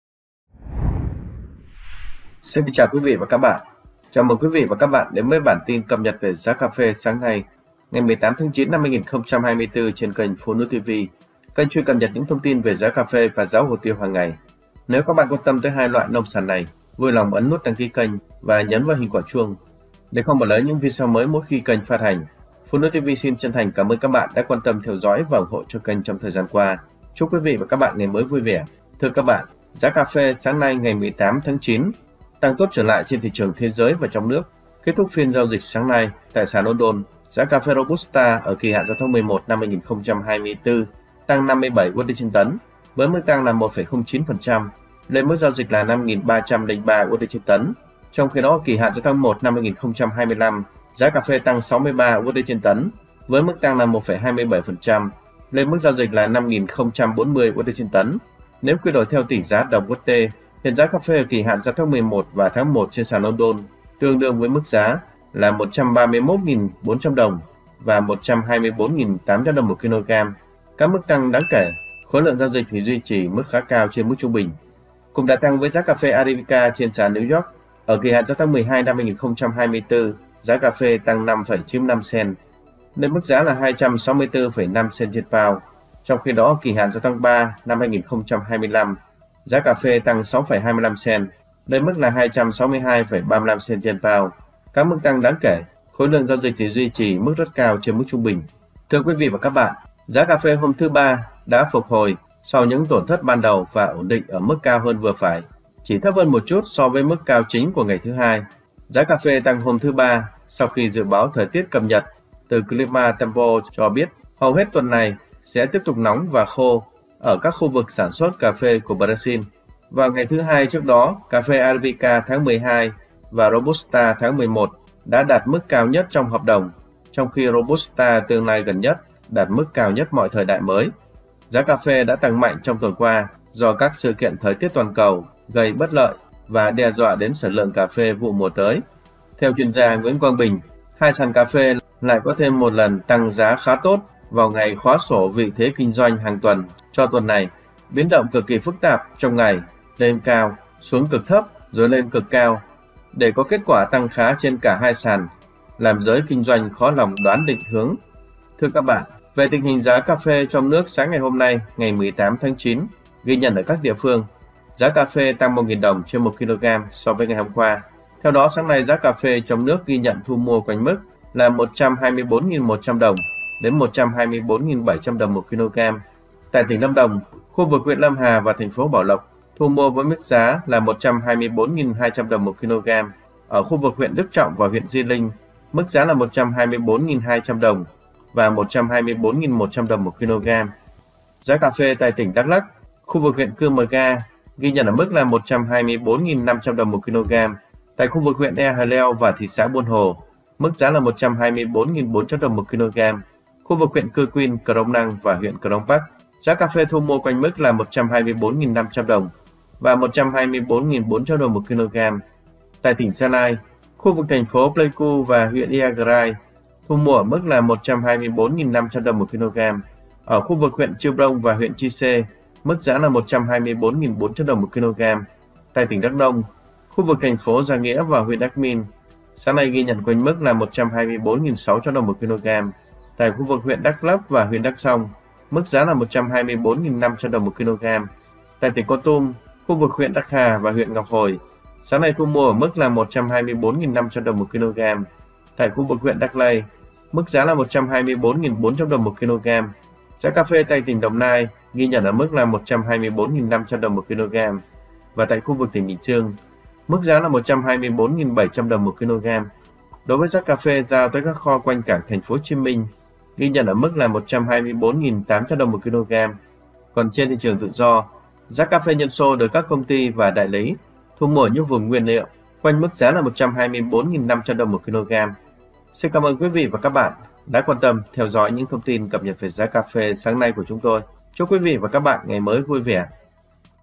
Giá cà phê hôm nay ngày 18/9/2024|Giá cà phê bật tăng trở lại ở 2 sàn và trong nước
👉 Bản tin cập nhật giá cà phê, hồ tiêu hôm nay ngày 18/9/2024 được chúng tôi cập nhật chi tiết, các thông tin trên thế giới và trong nước tại các vùng trồng trọng điểm khu vực Tây Nguyên và miền Nam. Giá chi tiết tại các địa phương như Đắk Lắk, Gia Lai, Kon Tum, Đắk Nông, Lâm Đồng, Đồng Nai, Bình Dương...
© Copyright by PhoNui TV ☞ Do not Reup